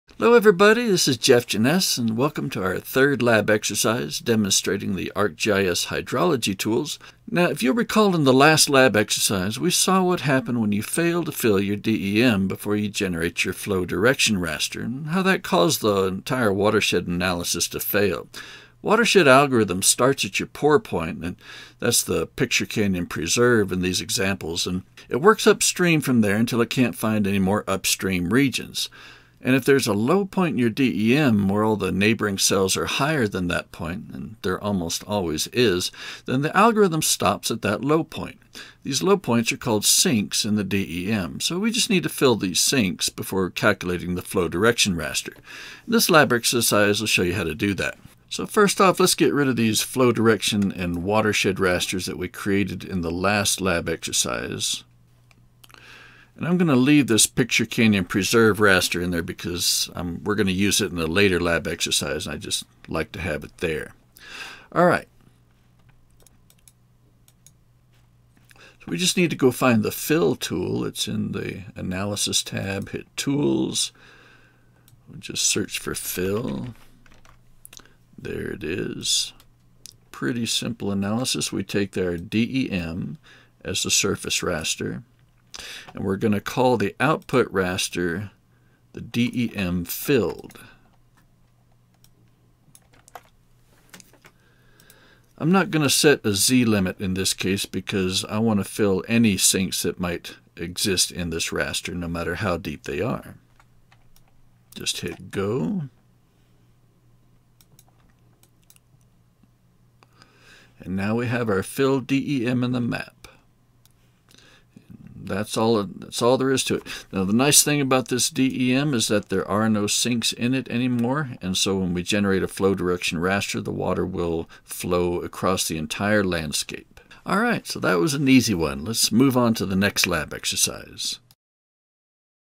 Hydrology in ArcGIS Pro, Lab Exercise 3: Filling Sinks in a DEM
In Lab Exercise 2 we saw what happens when you fail to fill your DEM before you generate your flow direction raster, and how that caused your watershed analysis to fail.  The watershed algorithm starts at your pour point (which is the Picture Canyon Preserve in these labs), and works upstream until it can't find any more upstream regions.  If there is a low point in the DEM where all the neighboring cells are higher than that point (and there almost always is), then the algorithm stops at that low point.  These low points are called sinks in the DEM.

So we just need to fill these sinks in the DEM before calculating the Flow Direction raster.  This lab exercise will show you how to do that, using the Fill tool.